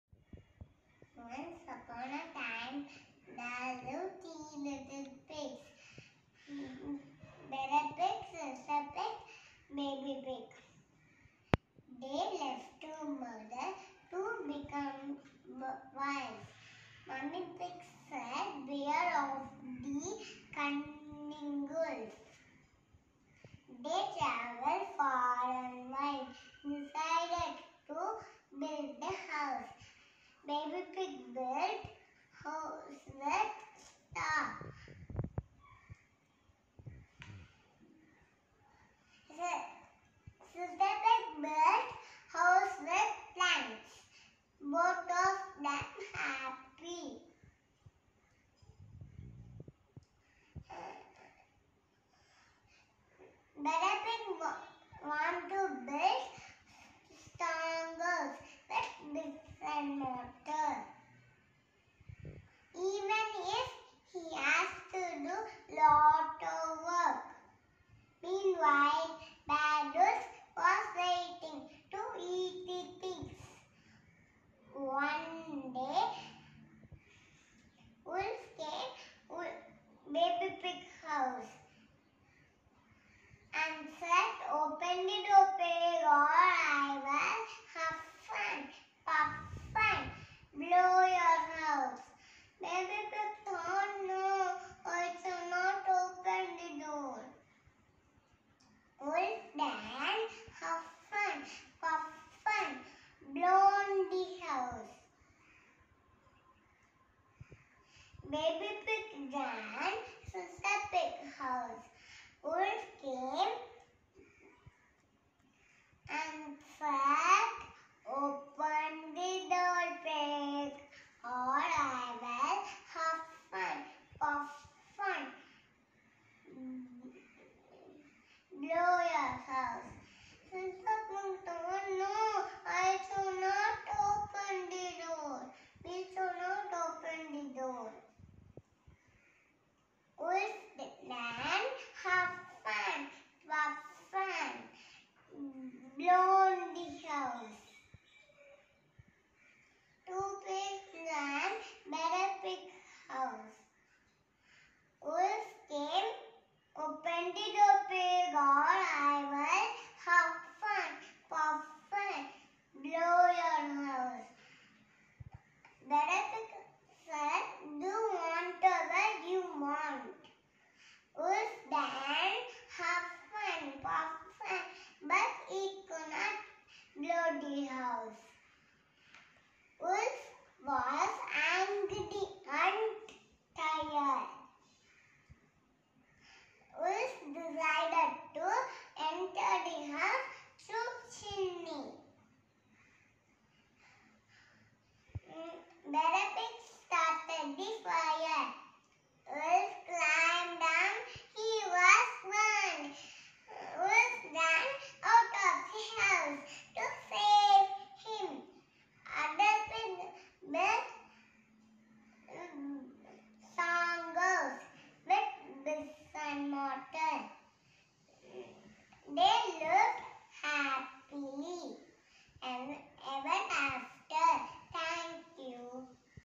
Three little pigs story
Charu telling a story about three little pigs and how the hard work of a pig saves them all from the wolf..